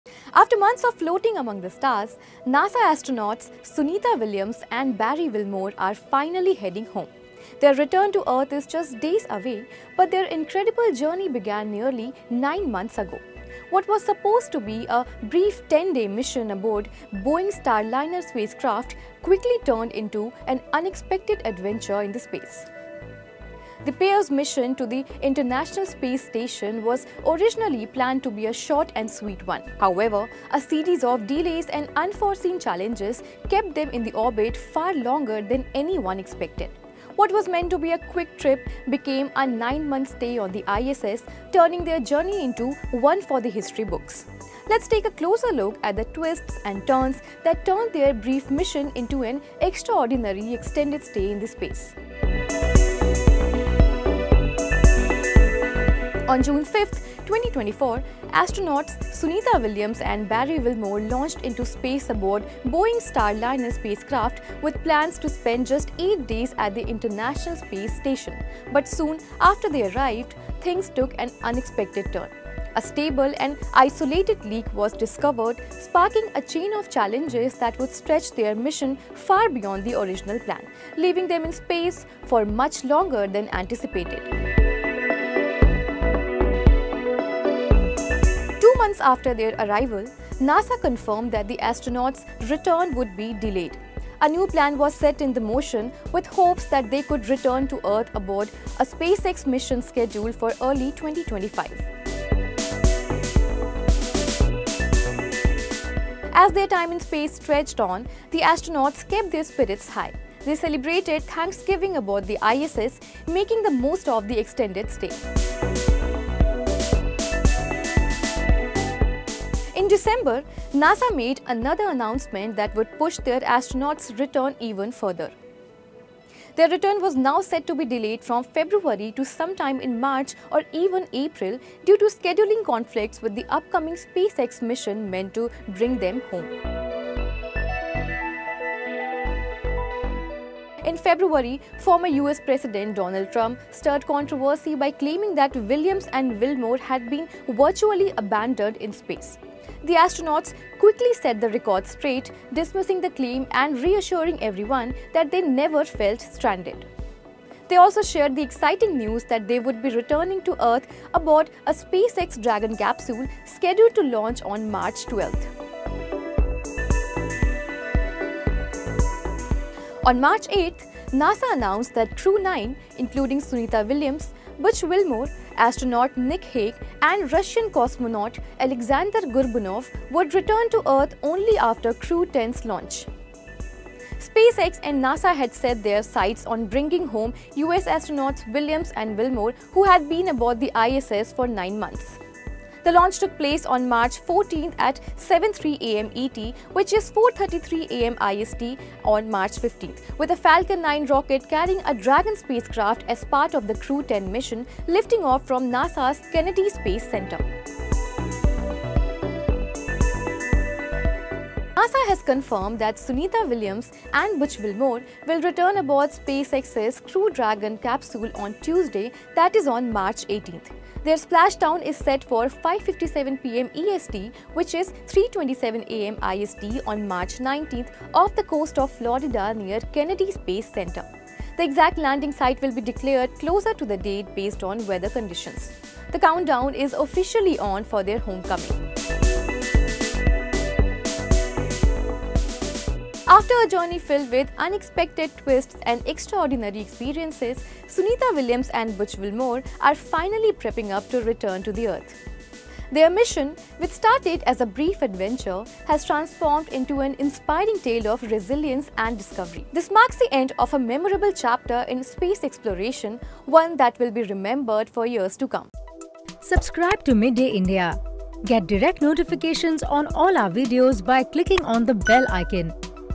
Sunita Williams & Barry Wilmore’s homecoming: How a short space trip became a 9-month saga
After months of floating among the stars, NASA astronauts Sunita "Suni" Williams and Barry "Butch" Wilmore are finally heading home. Their return to Earth is just days away, but their incredible journey began nearly nine months ago. What was supposed to be a brief 10-day mission aboard Boeing’s Starliner spacecraft quickly turned into an unexpected adventure in space. The pair’s mission to the International Space Station (ISS) was originally planned to be short and sweet. However, a series of delays and unforeseen challenges kept them in orbit far longer than anyone expected. What was meant to be a quick trip became a nine-month stay on the ISS, turning their journey into one for the history books. Let’s take a closer look at the twists and turns that turned their brief mission into an extraordinary extended stay in space.